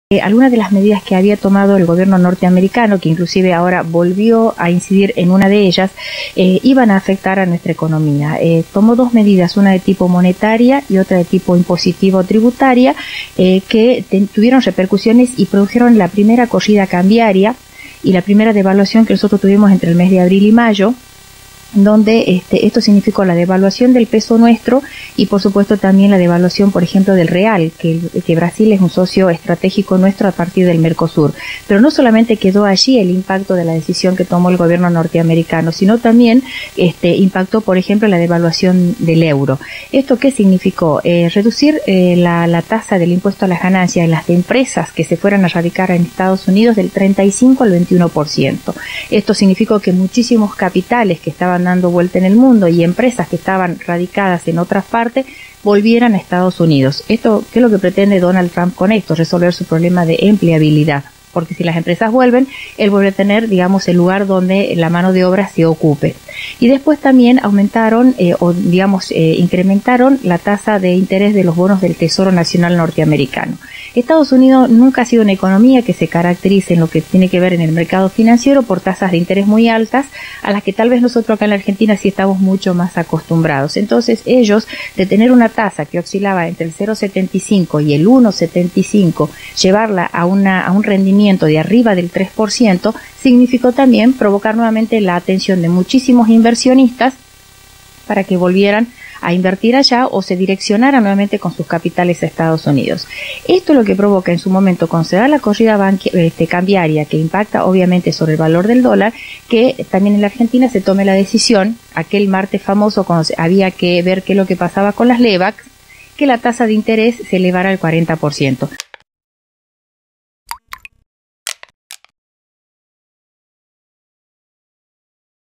Últimas Medidas Tributarias Y Cambiarias En Argentina
La divisa bajó a $28,10 en el Banco Nación y a $28,41 en el promedio de cotizaciones, en una rueda con marcada volatilidad. La entidad ahora comandada por Luis Caputo anunció el nuevo plan de contención del tipo de cambio.